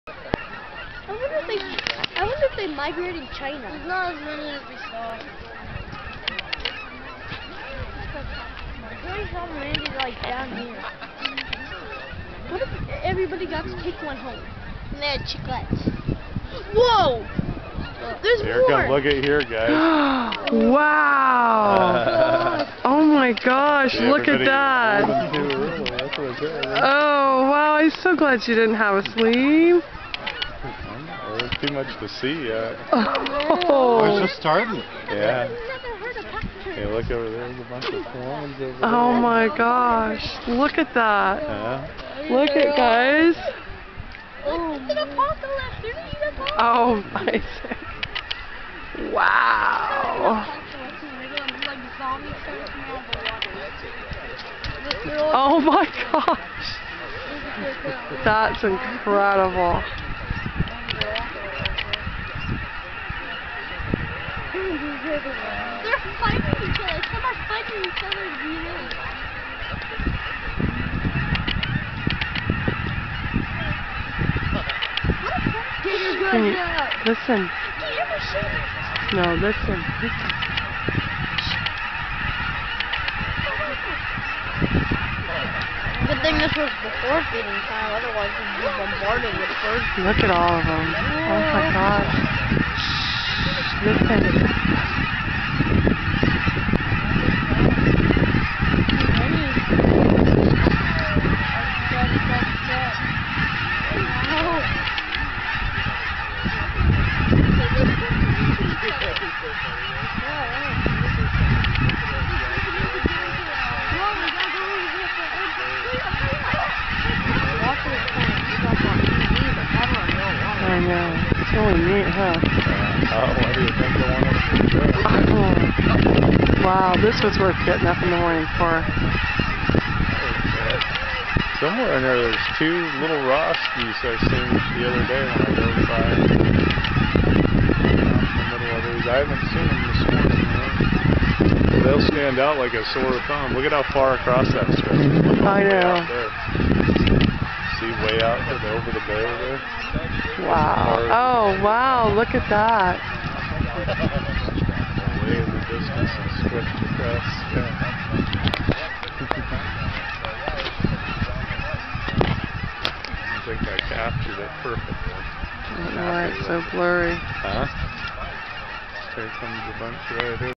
Goose Fly Off March 5 2011 015.MOV
Pack 47 Cub Scouts of Fortuna went to the Humboldt Wildlife Refuge to see the annual Goose Fly Off.  The kids had a blast.n  Thanks to the Friends of Humboldt Wildlife Refuge who put on the kid's activities and provided coffee and muffins.